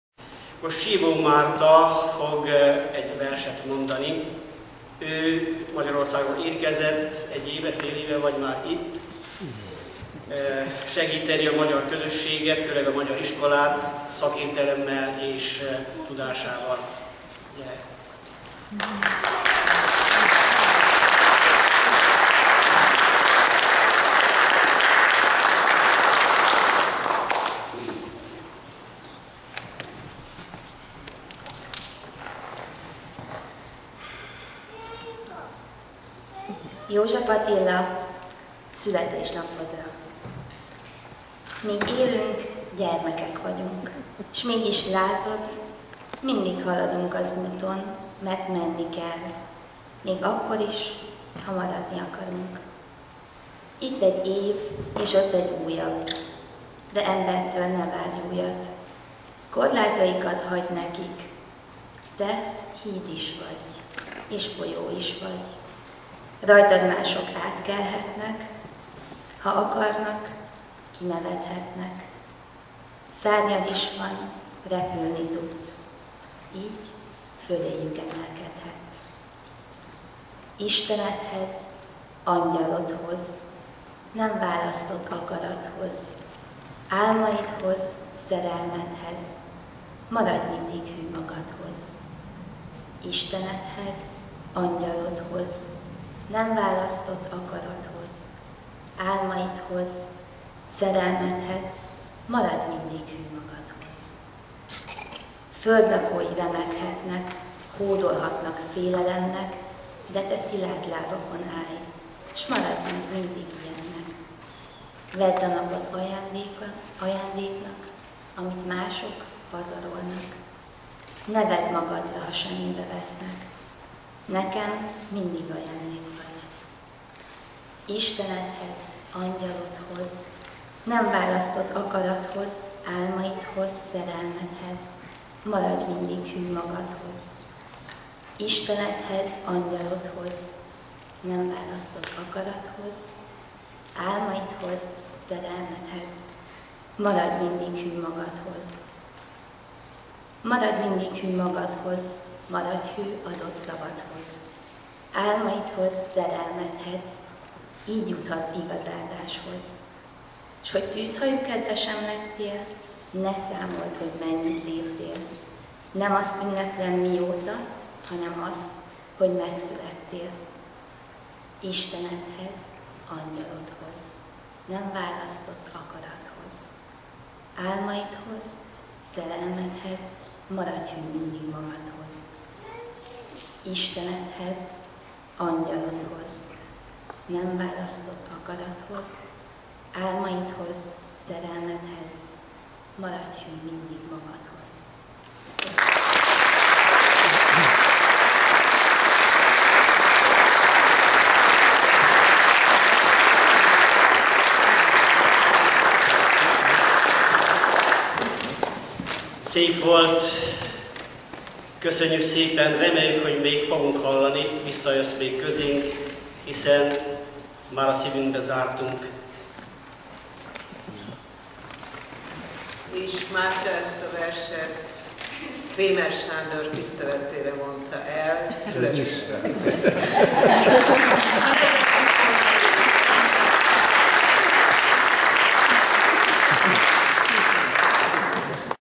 Klasszikus zenei koncert.
A Szabad Magyar Református Egyháznál
Csikágóban Sivó Márta szavalja József
 Attila Versét